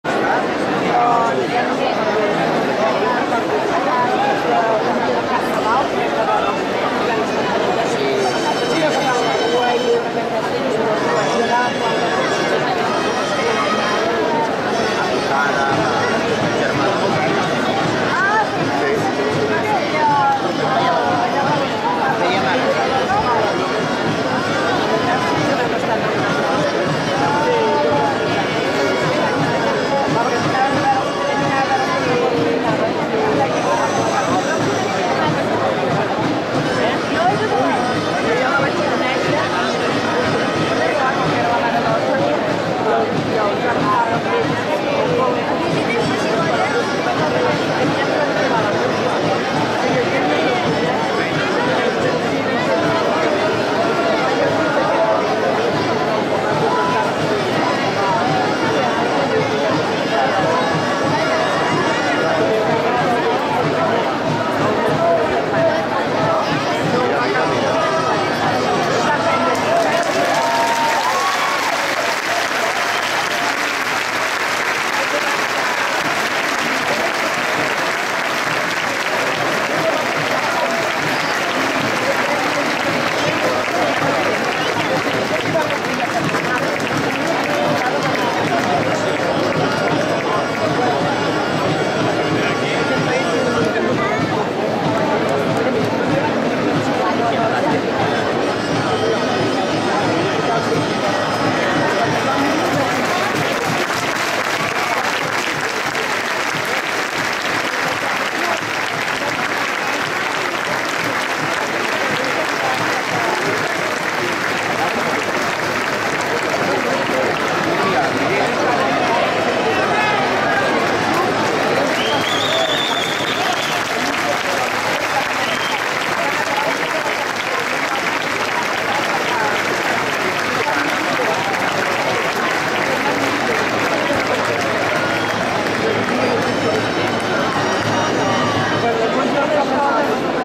2015-09-06. Capgrossos de Mataró, Torre de 8f, Sabadell
Diada Castellera de la Festa Major, Plaça Sant Roc amb Castellers de Sabadell "Saballuts", Castellers de Barcelona i Minyons de Terrassa